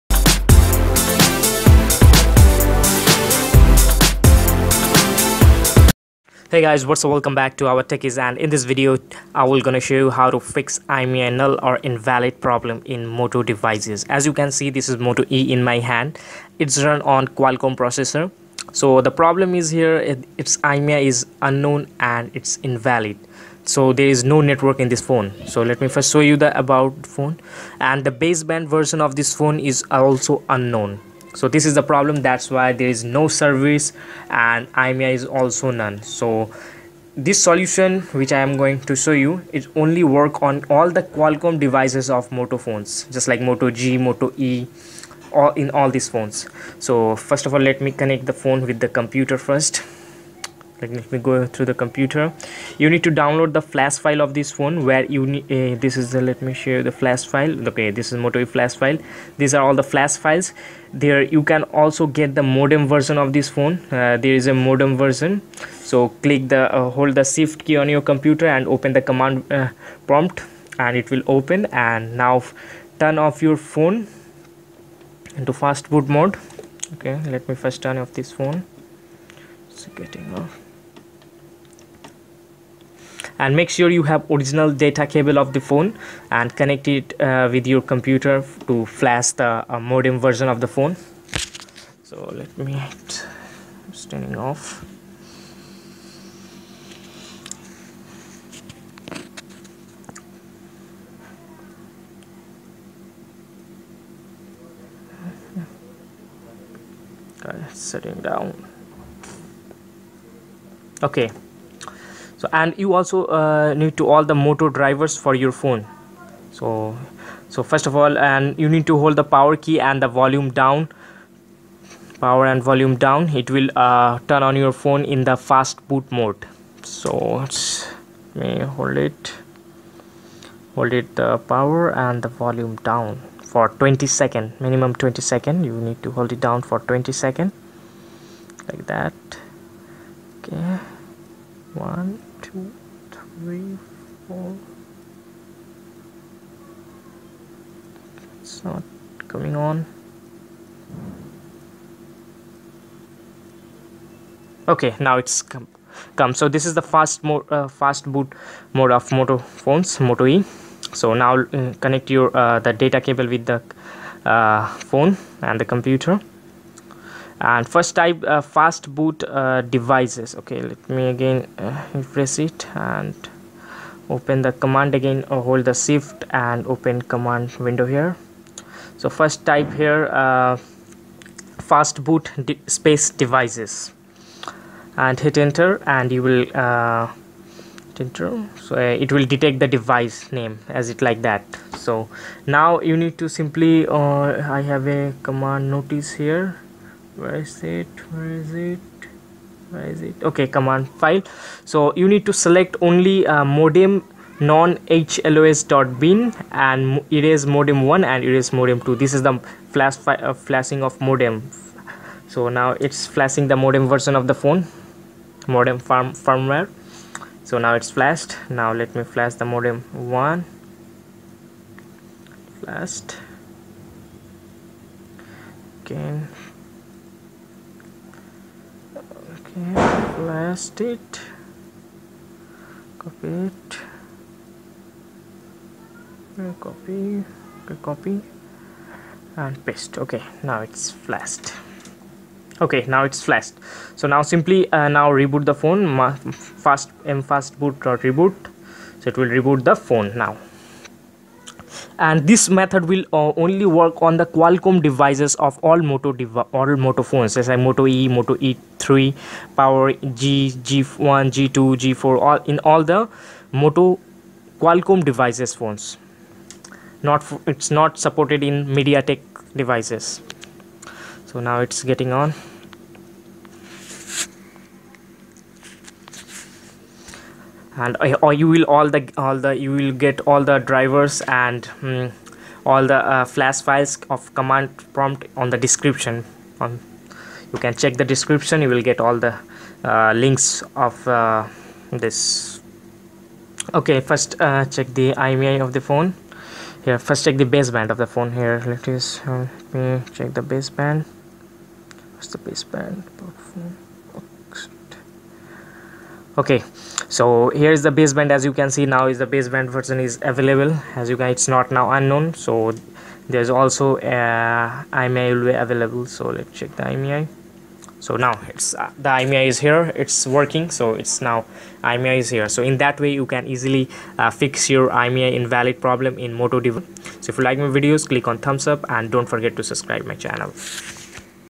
MOTO E\E2\E3\G\G2\G3\G4 PLUS IMEI NULL |INVALID||NO SERVICE||UNKNOWN BASEBAND PROBLEM FIXED
THIS VIDEO WILL HELP YOU TO FIX IMEI INVALID OR UNKNOWN BASEBAND PROBLEM IN ALL MOTO PHONES WITH QUALCOMM CHIPSET

3.  DOWNLOAD FLASH FILE OR MODEM FRMWARE OF YOUR PHONE
MODEM FIRMWARE IS ALWAYS DIFFRENT FOR DIFFERENT PHONE
 AND THEN DO THE PROCESSS FROM VIDEO AS IT IS

FOR LINK OF YOUR PHONE"S FLASH FILE
PLS ASK IN THE COMMENT BOX  WITH YOUR PHONE"S MODEL NUMBER